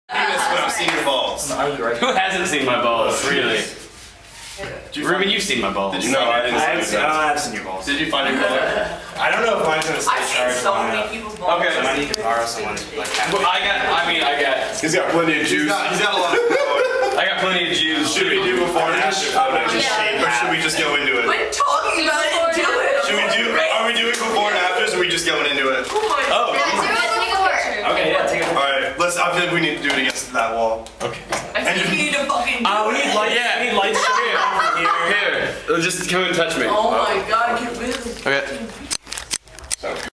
Chapter 1 Preparation for Shaving the Beards
Three men shaving their beards in a college dormitory bathroom in the middle of the night.